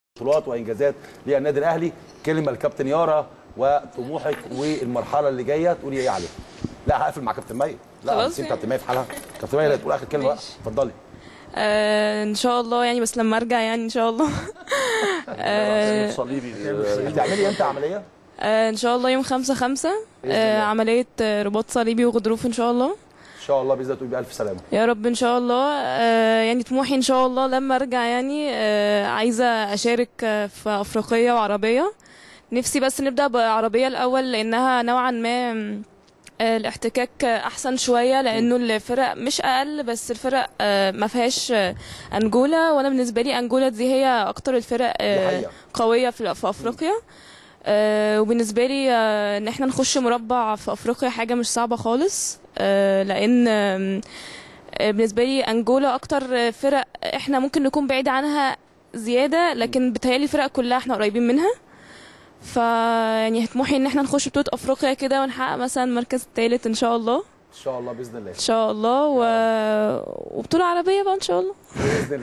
يارا شحاته تكشف عن خضوعها لعملية الرباط الصليبى بسبب اصاباتها خلال مباريات الفريق
التردد: 12341 أفقي.
القمر: نايل سات.
معامل الخطأ: 5/6.
معدل الترميز: 27500